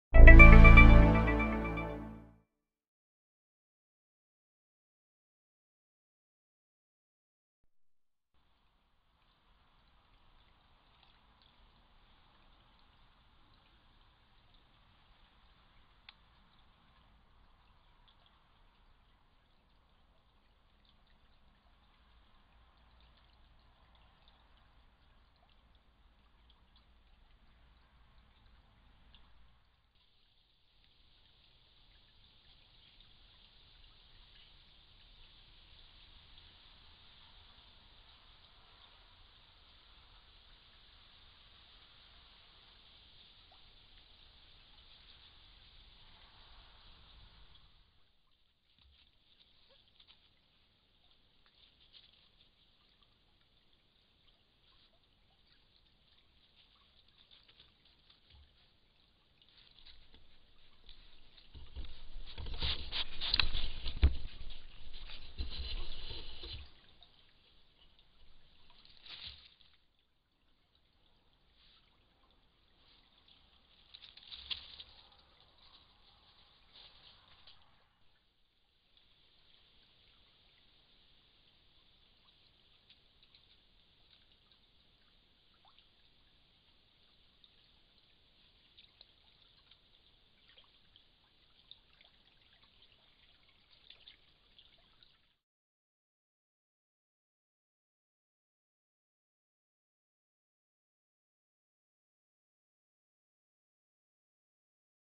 Otter, Mink & Fox by the River
Bushnell Trophy Cam footage of an otter, a mink and a fox, all using a secluded part of the river bank over a period of 3 nights.  No bait was used to lure the animals to the site.